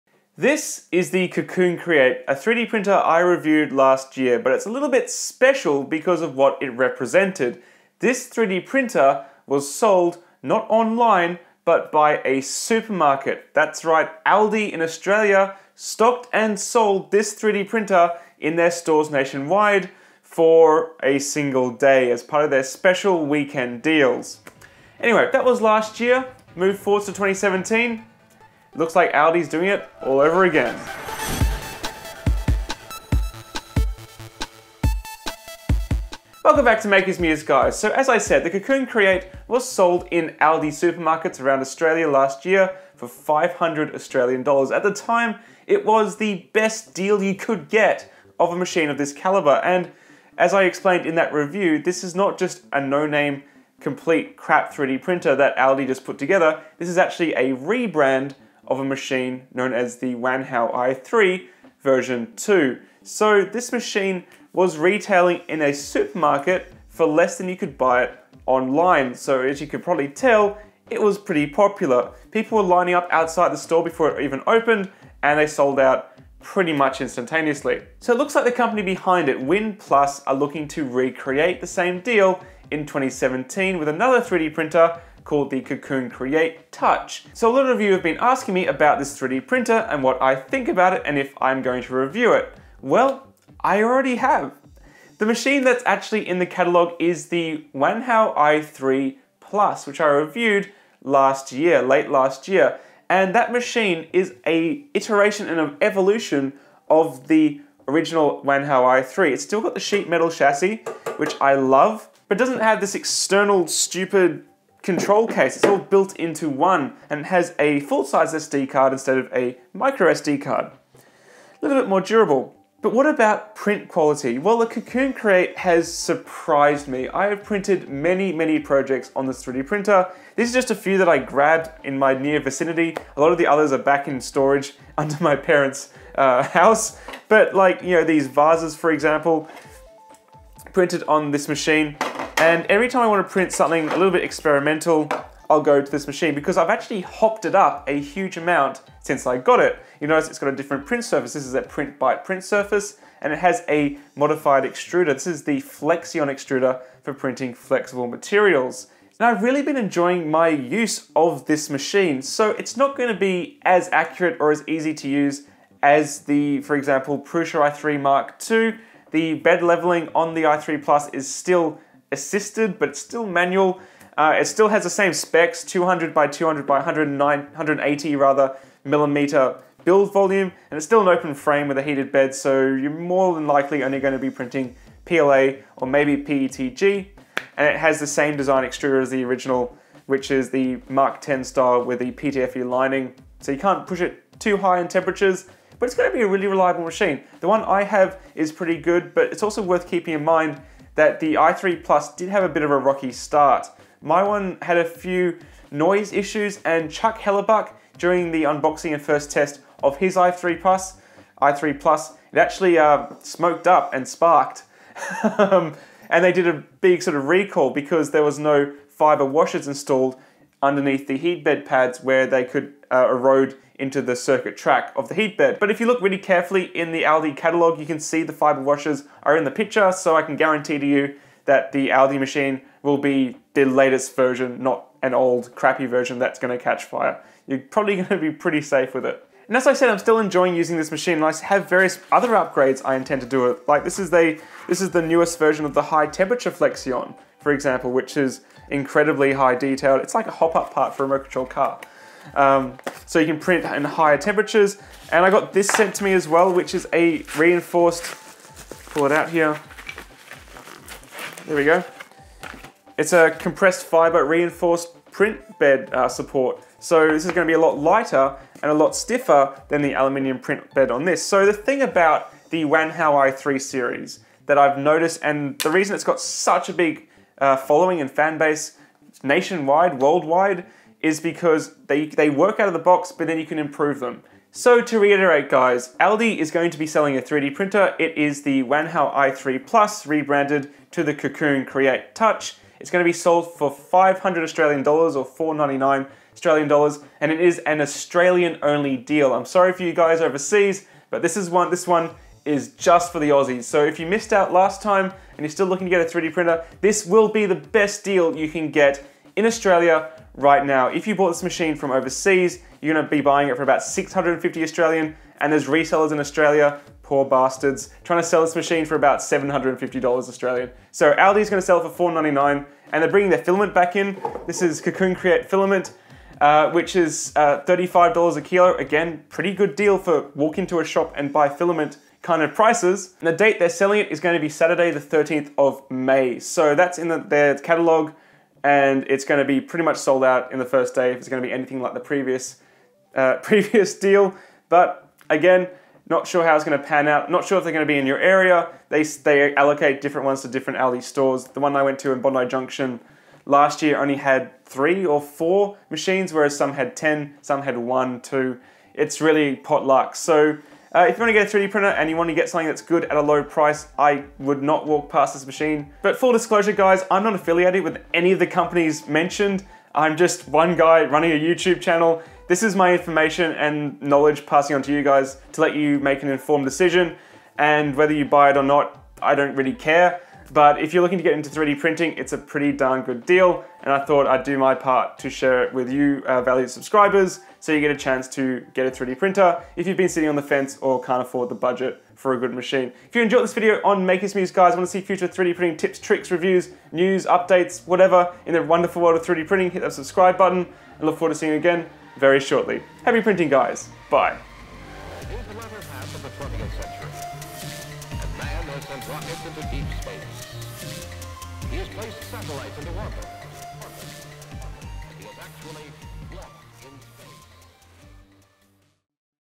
ALDI to sell 3D Printers... again!
Aldi supermarkets will be selling the Cocoon Create Touch 3D Printer on the 13th of May 2017 in Australia. Here's what you should know!